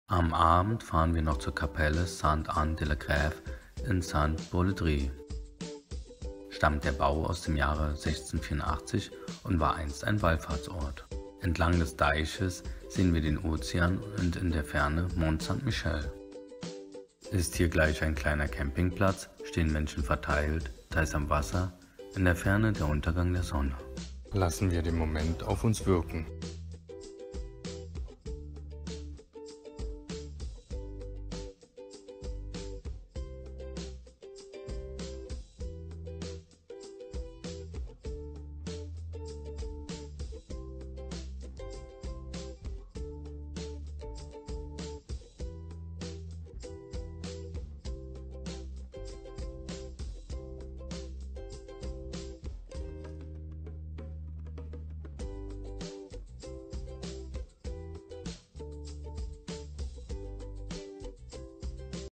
Frankreich - Bretagne - Saint Broladre - Kapelle Sainte Anne de la Grève - Sonnenuntergang 20.05.23
Frankreich - Bretagne - Saint-Broladre - Kapelle Sainte Anne de la Grève - Sonnenuntergang

Am Abend fahren wir noch zur Kapelle Sainte Anne de la Grève in Saint-Broladre.
Stammt der Bau aus dem Jahre 1684 und war einst ein Wallfahrtsort.

Entlang des Deiches sehen wir den Ozean und in der Ferne Mont-Saint-Michel.
Ist hier gleich ein kleiner Camping Parkplatz stehen Menschen verteilt, teils am Wasser, in der Ferne der Untergang der Sonne.

Lassen wir den Moment auf uns wirken.

(fälschlicherweise steht im Video Normandie, die Kapelle steht aber schon in der Bretagne)